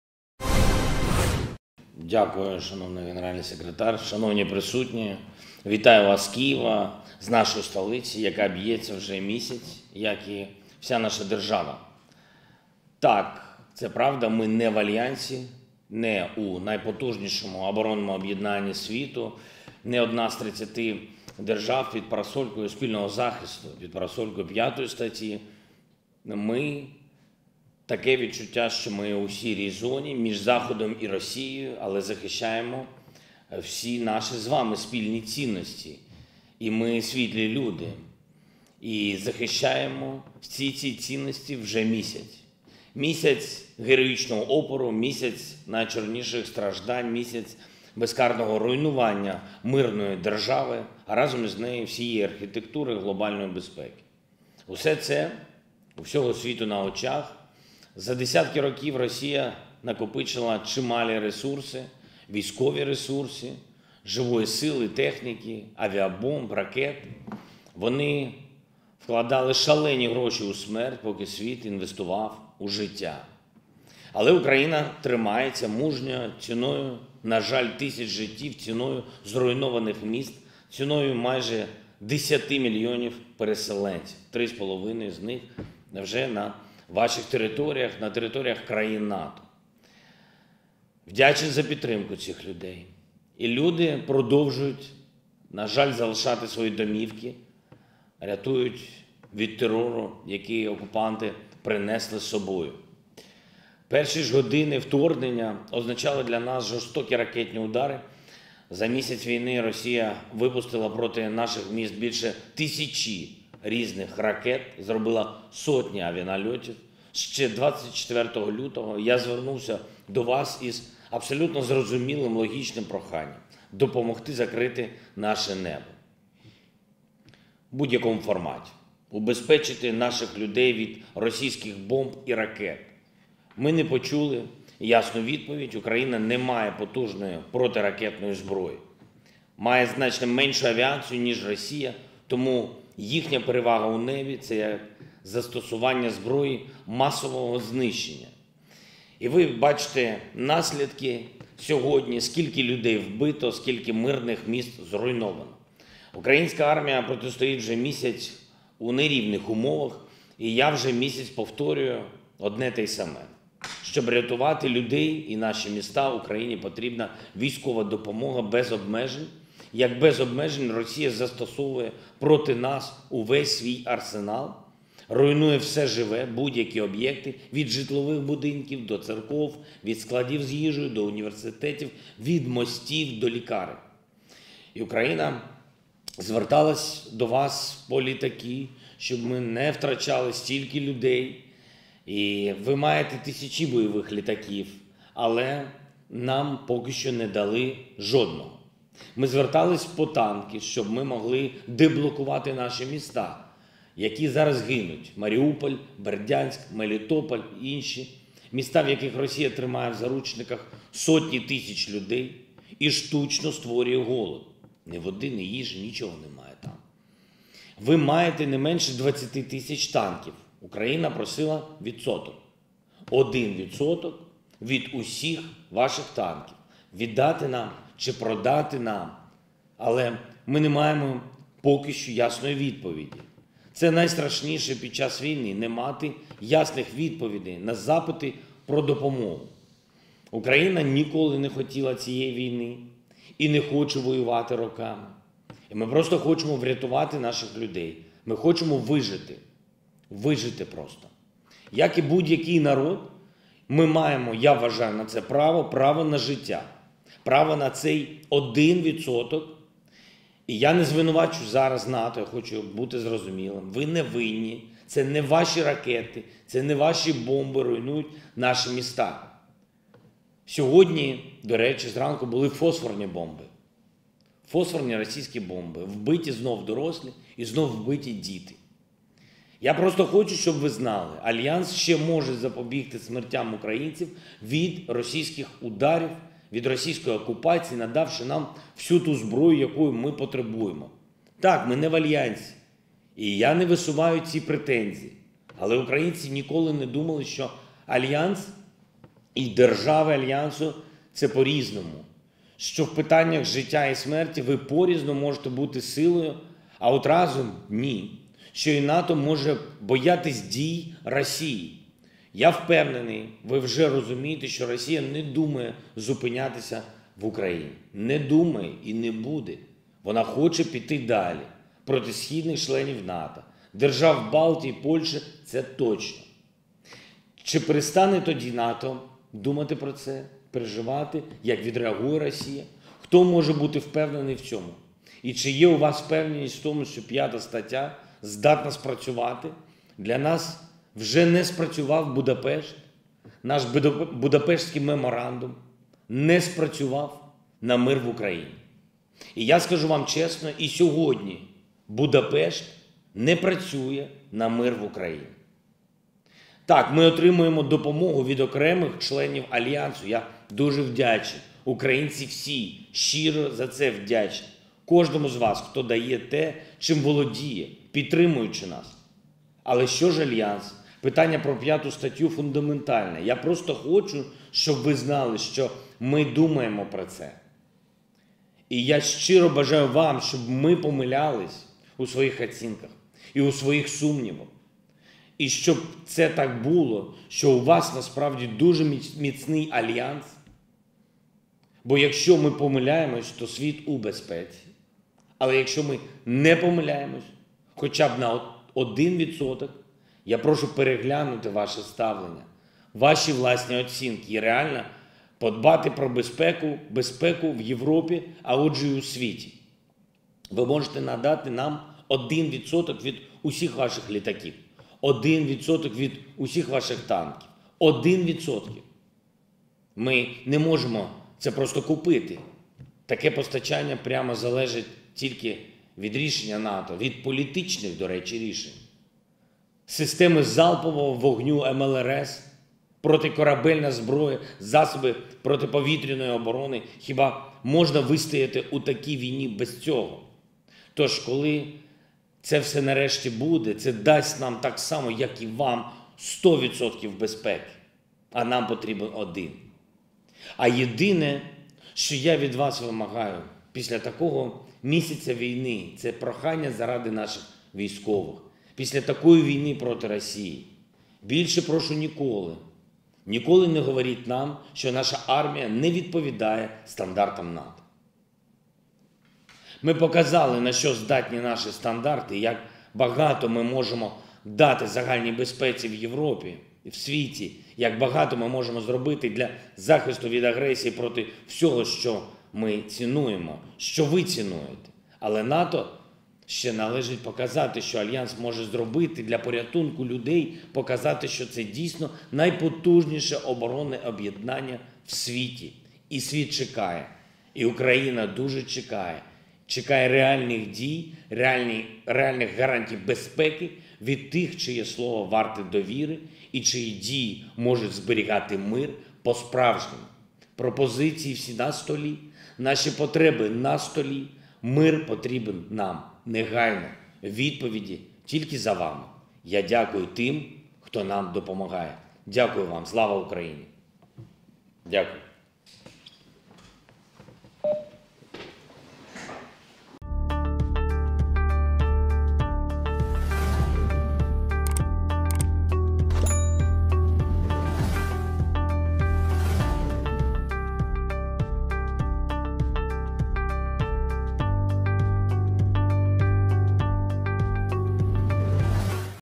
‼️🇺🇦Владимир Зеленский выступил на саммите НАТО
"Больше никогда, прошу вас, никогда не говорите нам, что наша армия не отвечает стандартам НАТО. Мы показали, на что способны наши стандарты. И как много мы можем дать общей безопасности в Европе и мире" - Зеленский